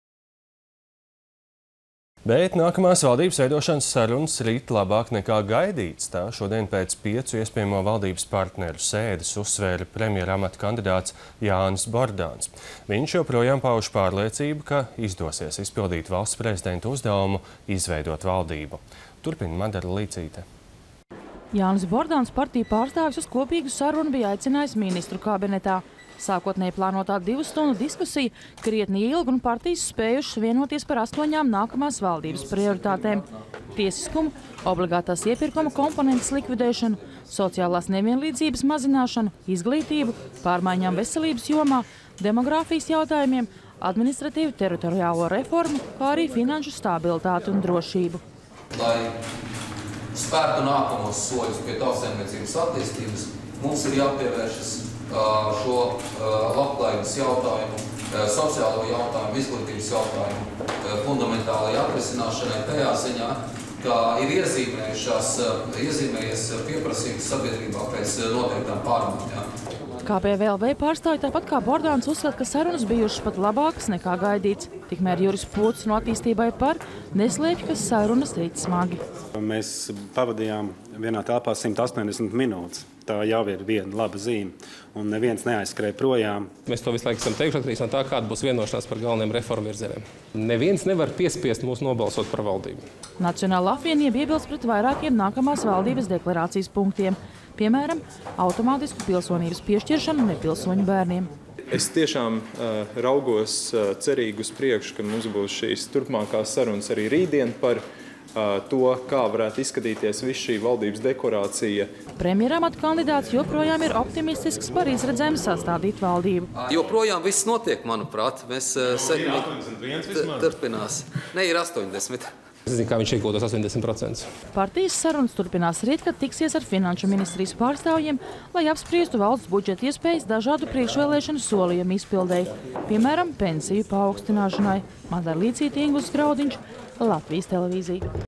J. Bordāns optimistisks par valdības apstiprināšanu
Nākamās valdības veidošanas sarunas rit labāk, nekā gaidīts. Tā šodien pēc piecu iespējamo valdības partneru sēdes uzvēra premjera amata kandidāts Jānis Bordāns. Viņš joprojām pauž pārliecību, ka izdosies izpildīt Valsts prezidenta uzdevumu – izveidot valdību. Turpina Madara Līcīte.